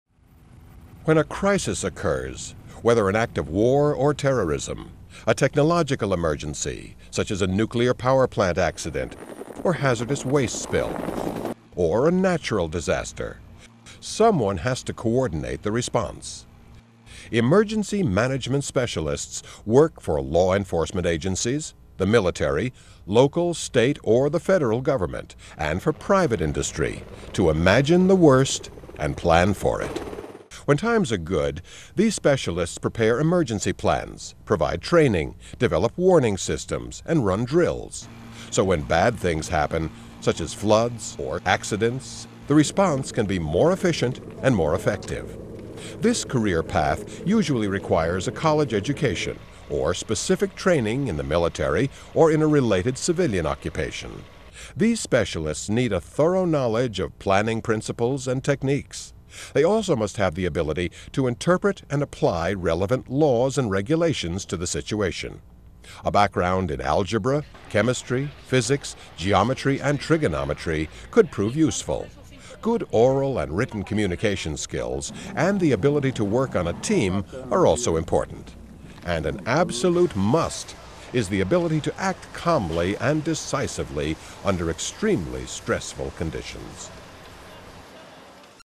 Science Career: Emergency Management Specialists
Overview of Emergency Management Specialists careers from the Department of Labor and Workforce Development. For use in Science Buddies Career Profiles.